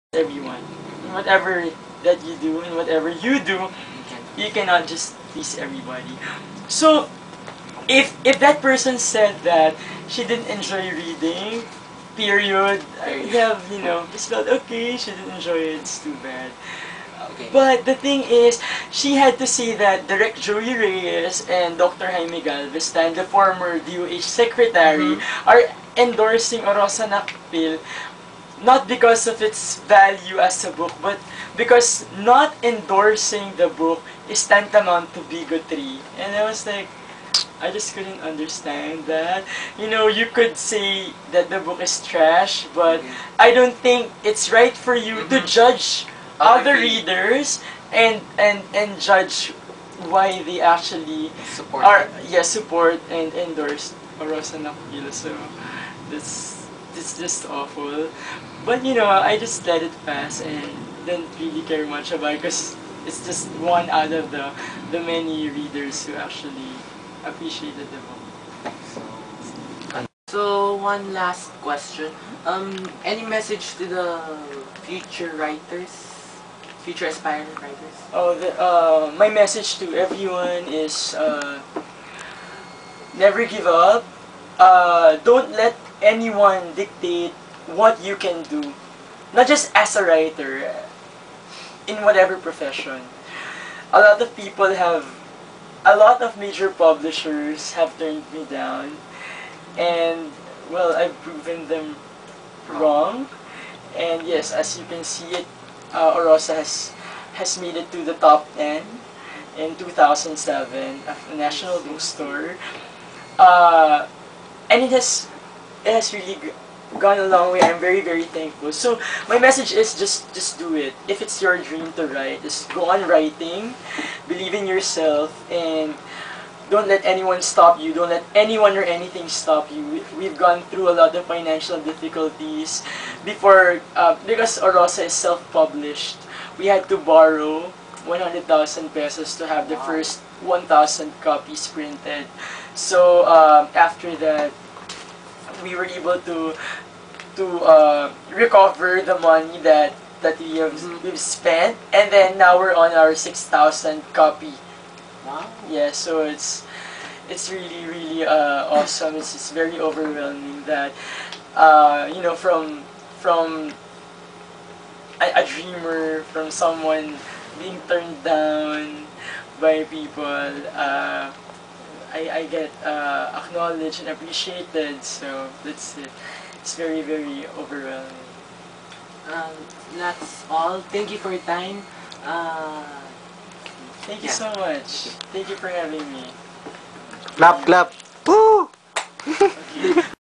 Interview with Mr. Louie Mar A. Gangcuangco part 3 of 3
Ateneo High School
4th year English Project
Spotlight On the Filipino Author

Author featured:
Mr. Louie Mar A. Gangcuangco
Works:
Orosa Nakpil-Malate
Gee! My Grades Are Terrific!